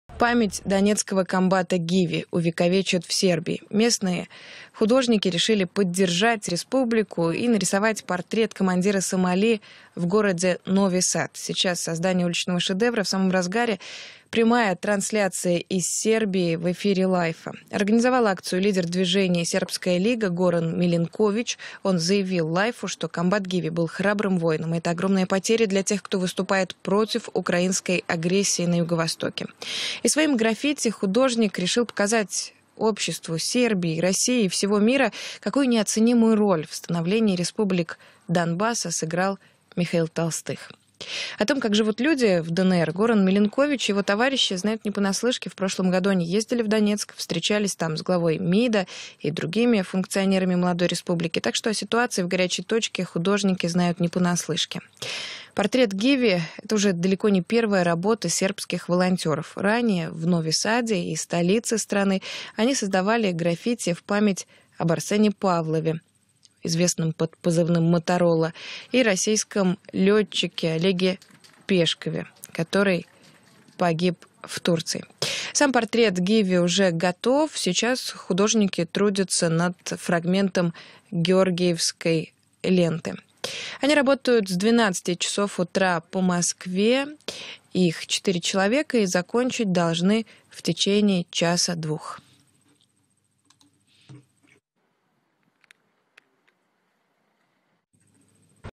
Сербские граффитисты почтут память комбат “Сомали” в Нови-Саде
Память донецкого комбата Гиви увековечат в Сербии